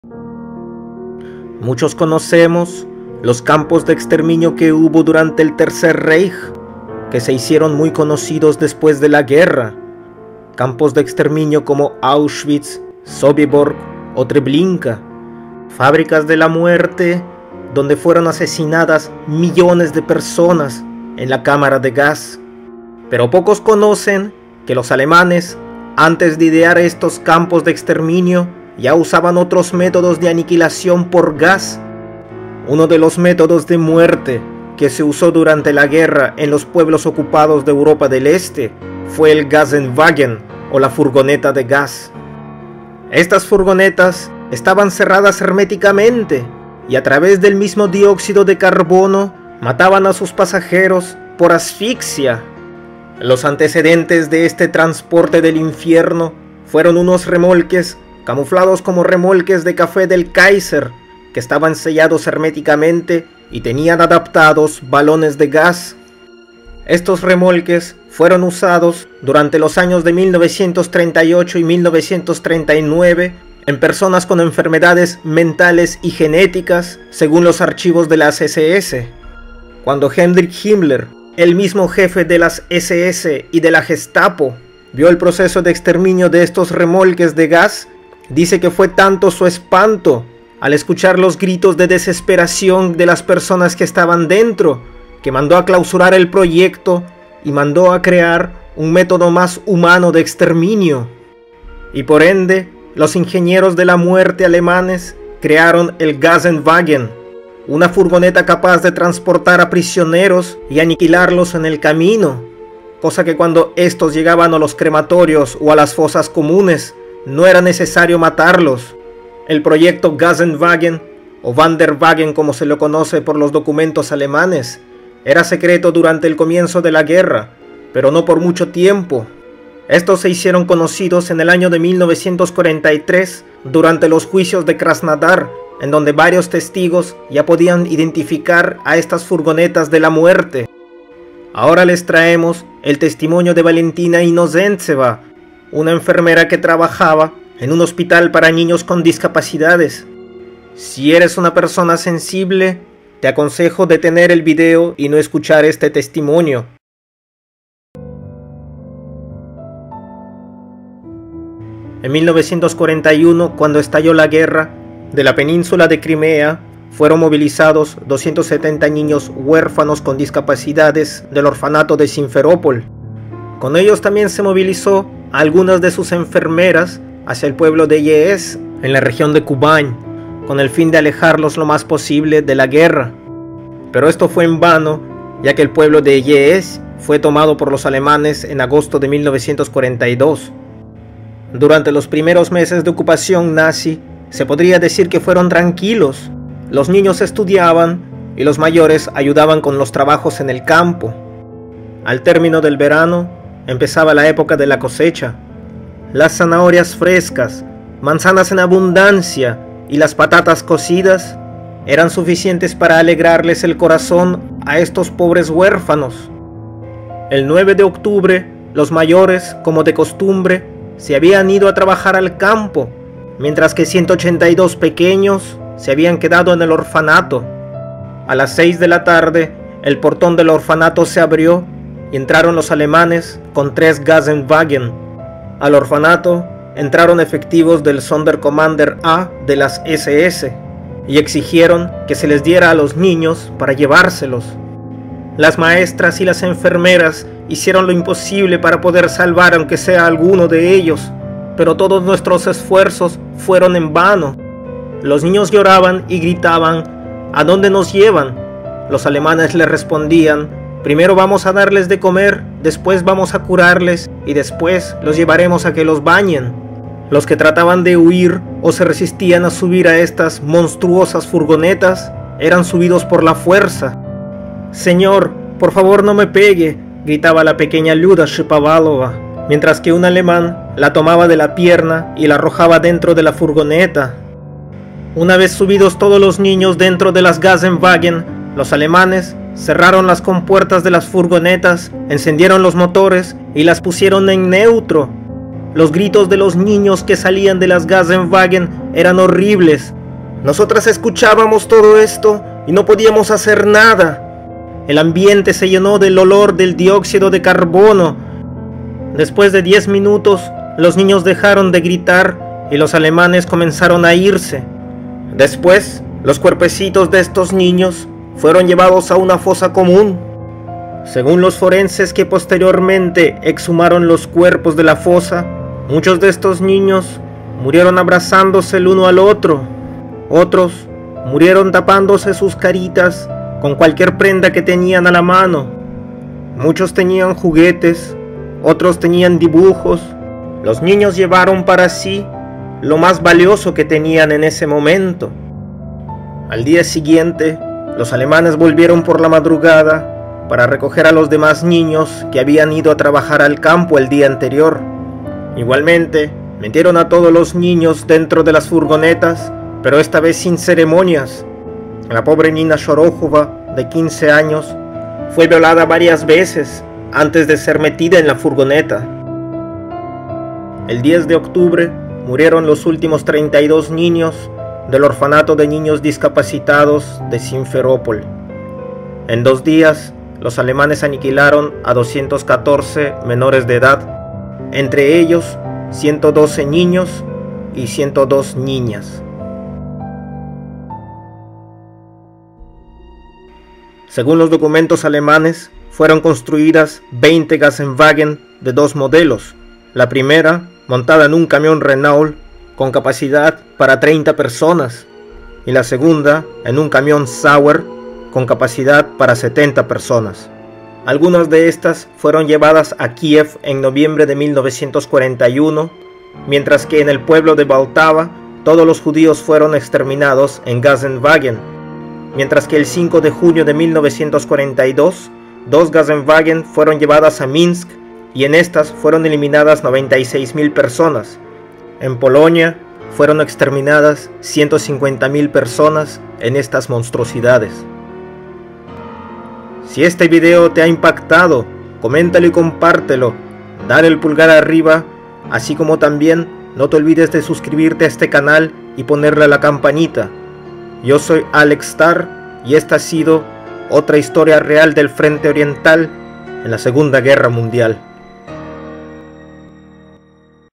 FRENTE ORIENTAL - Testimonio de Valentina I. del exterminio de los niños del orfanato de Sinferópol
Entérate de esto y mucho más en nuestro canal de Telegram
Muchos conocemos que los alemanes aniquilaron a millones de personas en los campos de exterminio usando cámaras de gas. Pero pocos conocen que antes del uso de las cámaras los alemanes ya aniquilaban a personas por otros métodos de gas.

Entérate de cómo se aniquilaban a los civiles en Europa del este a través de una de las formas más monstruosas de muerte, el gazenwagen. A través del testimonio de Valentina Inozéntseva, una enfermera del hospital de niños con discapacidades del orfanato de Simferópol.

Muchas gracias!